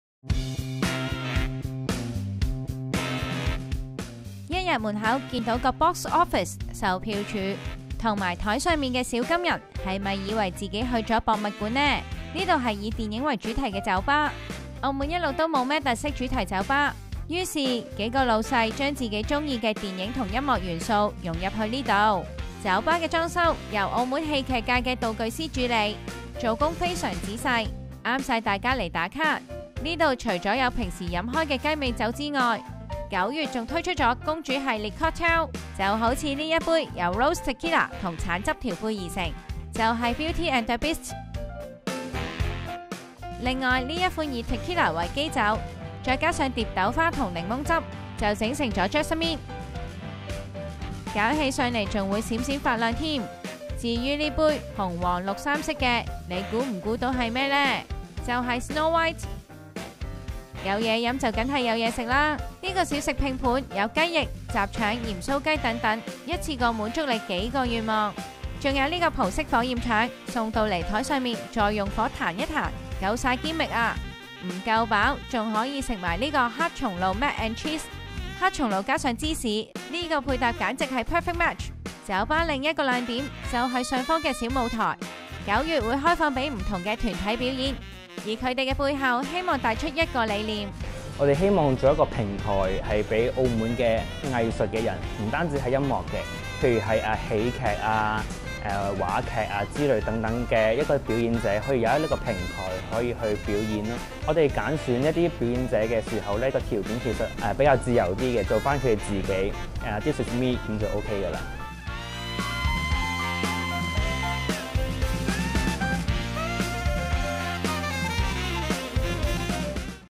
【快閃澳門】澳門新開電影主題酒吧 6款 Cocktail 小食必試推介
每次去澳門，街上好似冇咩留意有酒吧。呢間新開嘅特式主題酒吧絕對係「電影迷」嘅落腳點！酒吧裝修注入電影元素，入面唔少位置都夠晒比你打卡呃like， 9月新推出嘅雞尾酒系列，更加滿足大家的「少女心」。

【info】
Cinebrew Bar
地址：澳門林茂海邊大馬路金灣濠庭地下 A 鋪
營業時間：7pm - 4am
全方位緊貼《U Magazine》社交平台